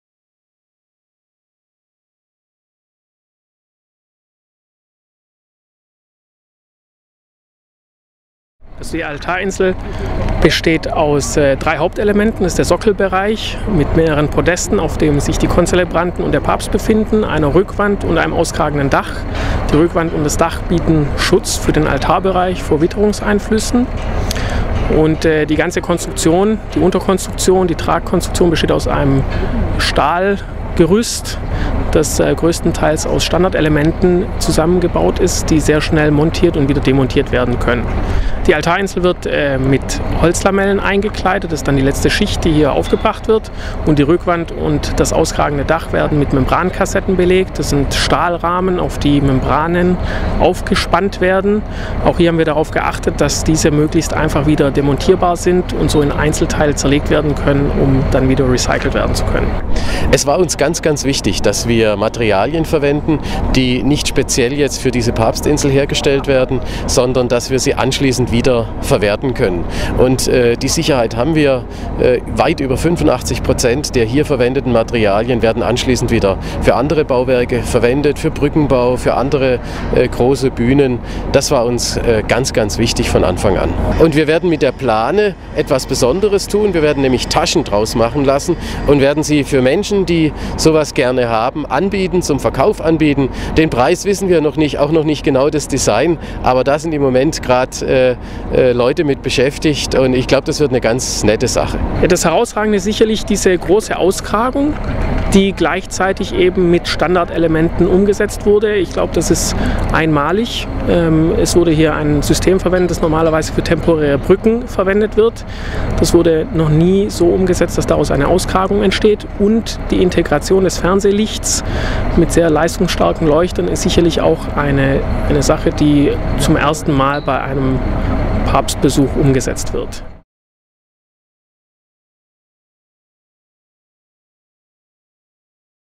Umweltfreundliche Architektur: Aufbau der Altarinsel auf dem Freiburger Flugplatzgelände
Ein ganz besonderes Highlight der Deutschlandreise von Papst Benedikt XVI. wird der Abschlussgottesdienst am 25. September auf dem Freiburger Flugplatzgelände sein und die Vorbereitungen für dieses wichtige Ereignis laufen bereits seit Wochen auf Hochtouren. Zurzeit wird die rund 2.000 Quadratmeter große Altarinsel aufgebaut mit ihrem beeindruckenden frei tragenden Dach.